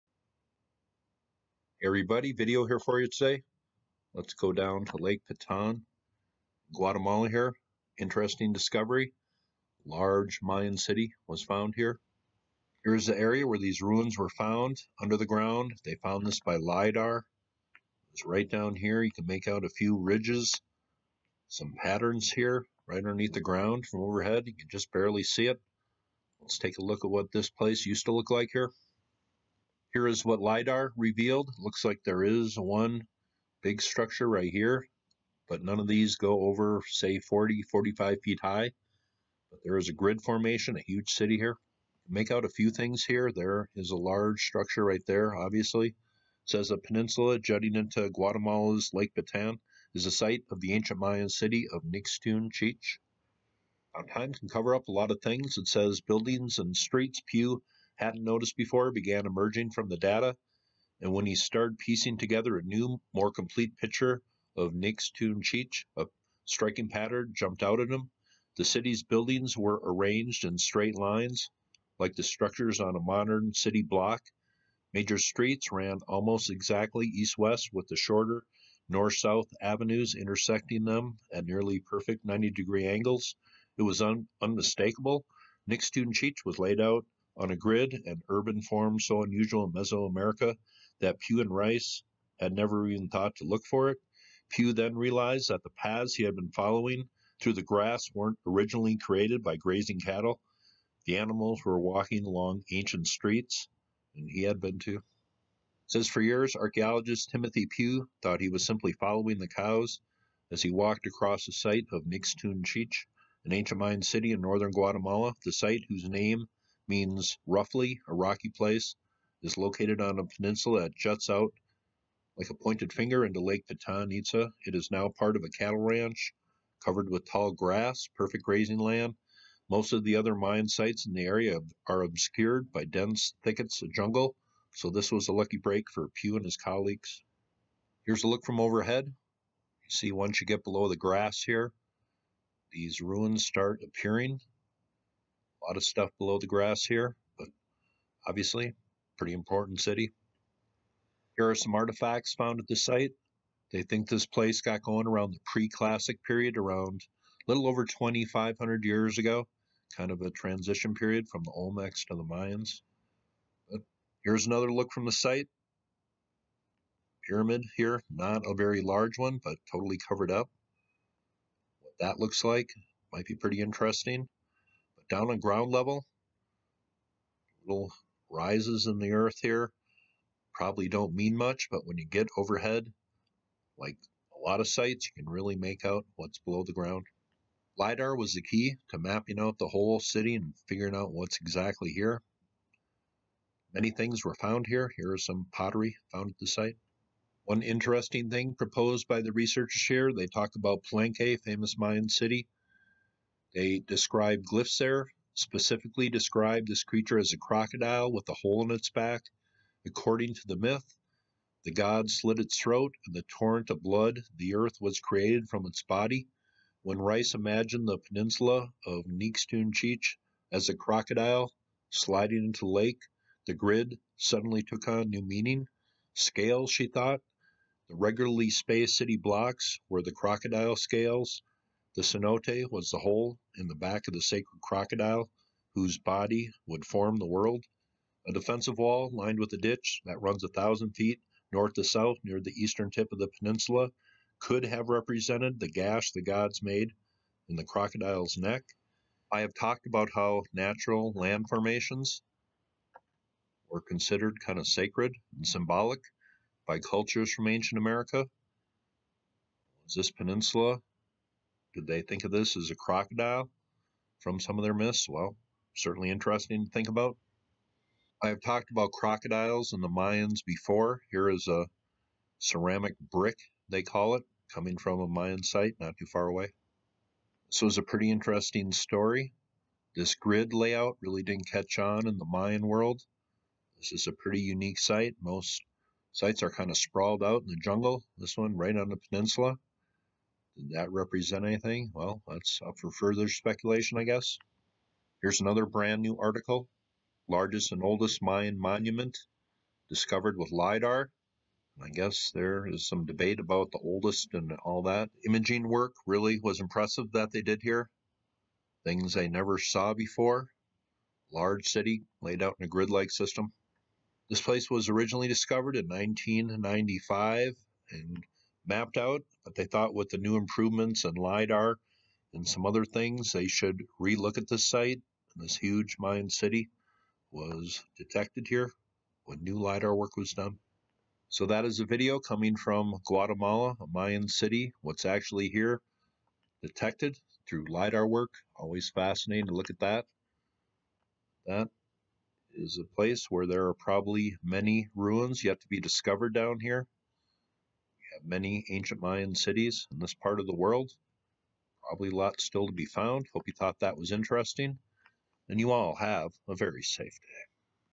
LiDAR Reveals Unique Mayan Grid City In Guatemala
New story.  I report on the Mayan city of Nixtun Ch’ich’, first mapped in 1995 but new LiDAR work has revealed a large unique city grid pattern on a peninsula on the shores of Lake Peten Itza in Guatemala.

This is a site In Mexico. This story popped up at the same time.